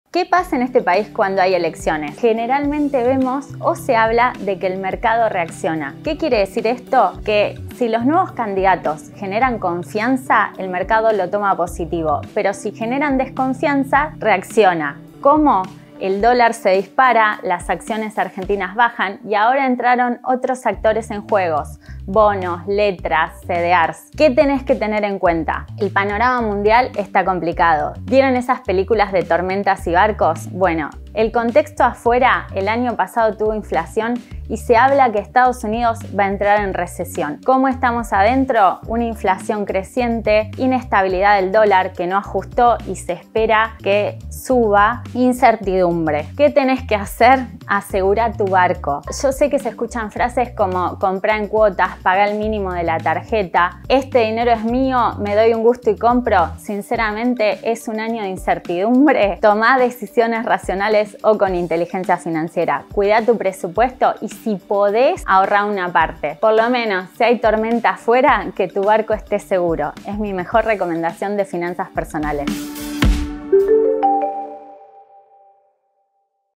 Elecciones y economía: cómo afectan los cambios de gobierno
Inflación, dólar, sequía, elecciones. Este 2023 viene movido y nos trae algunos recuerdos, generando incertidumbre. ¿Qué decisiones tenemos que tomar?